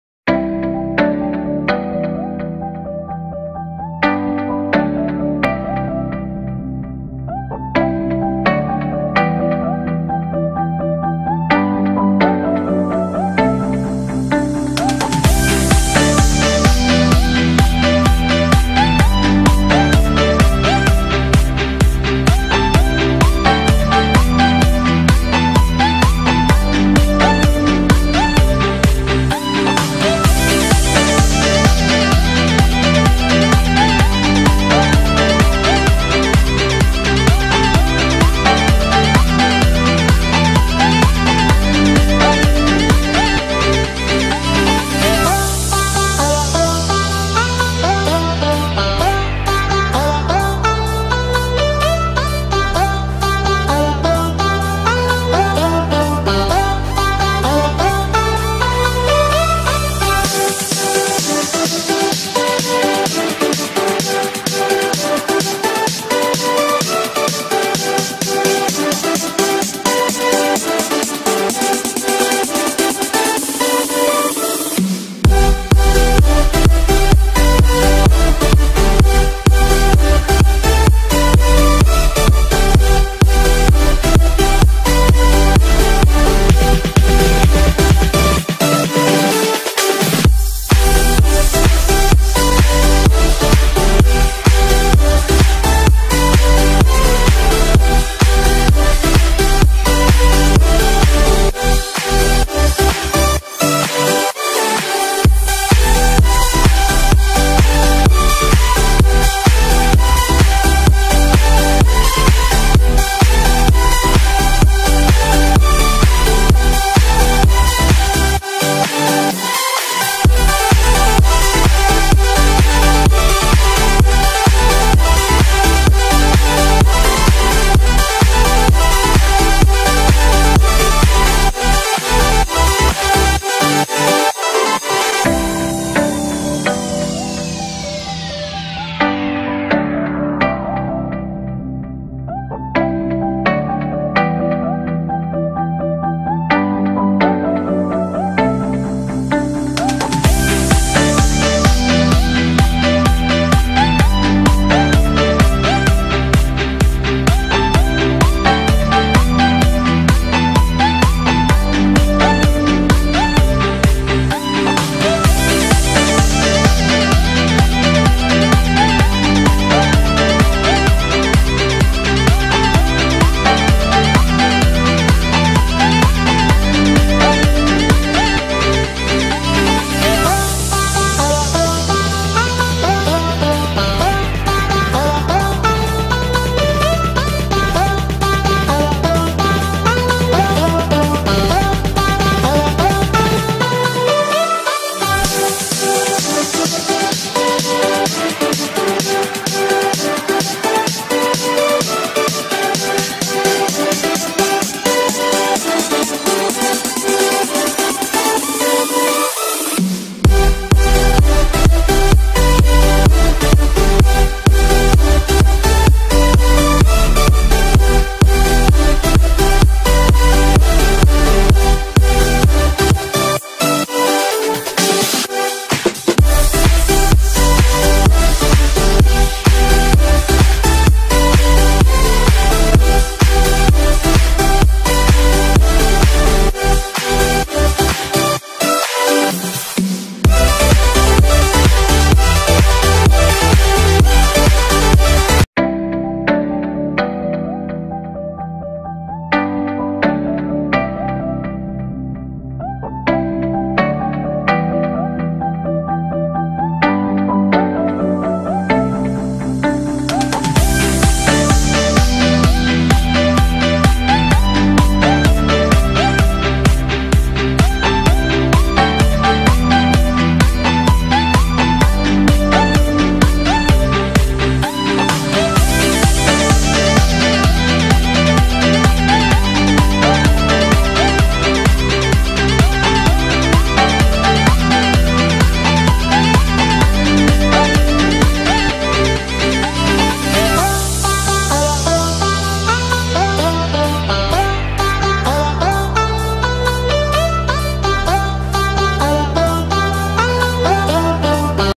pakistani chicken karahi recipe restaurant style | Cholistani chicken kadahi
Pakistani famous chicken kadahi recipe simple and easy
   Without onion, really delicious authentic traditional aromatic flavourful recipe 😋😋 must try at ur home